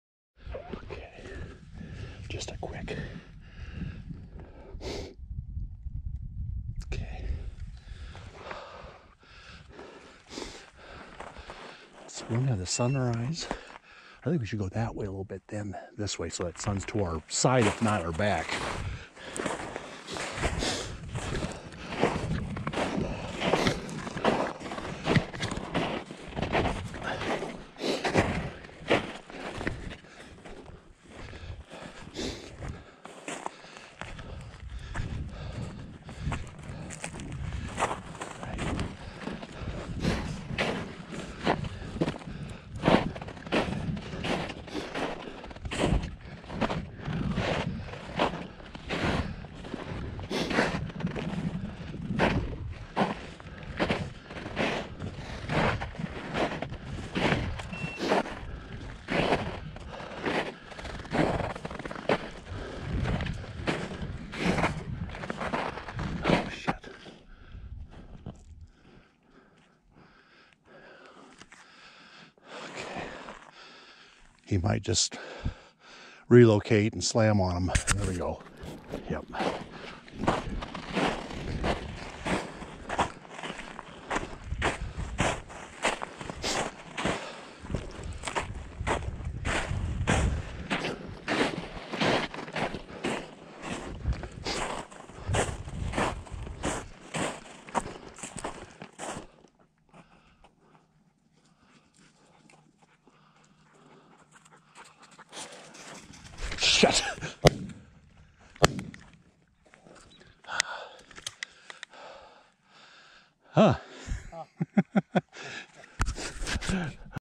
Runnin' Single 1-26-2024 : Wyoming Upland & English Setters
A lonely single runnin' Hun out in the Big Empty. Mitch snapped off a couple of shots but no bird down. A bit more "run' with the Setters then I like but. the dog(s) need to be where the birds "are"...not where they "were" R-E-L-O-C-A-T-E